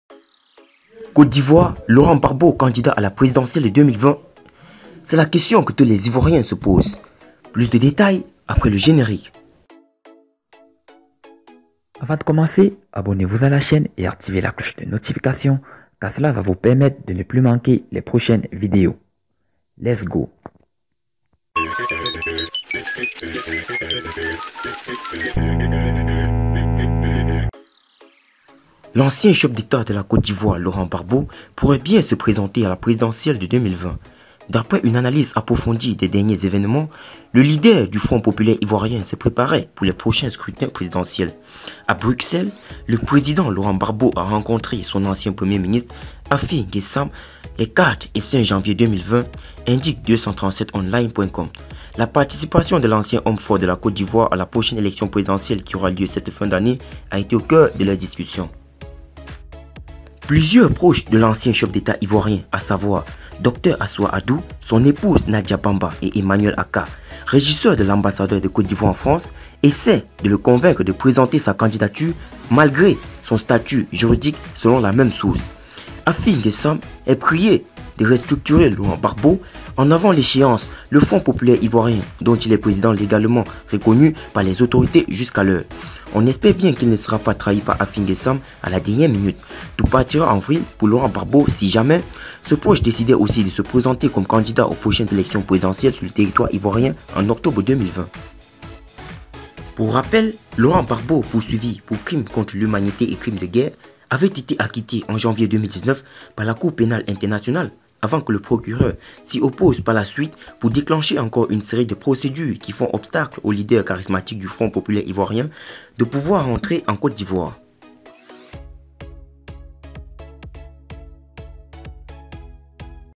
Côte d’Ivoire: Laurent Gbagbo candidat à la présidentielle 2020 en Côte d’Ivoire
L’ancien chef d’Etat de la Côte d’Ivoire, Laurent Gbagbo pourrait bien se présenter à la présidentielle 2020. D’après une analyse approfondie des derniers événements, le leader du Front populaire ivoirien se préparait pour les prochains scrutins présidentiels.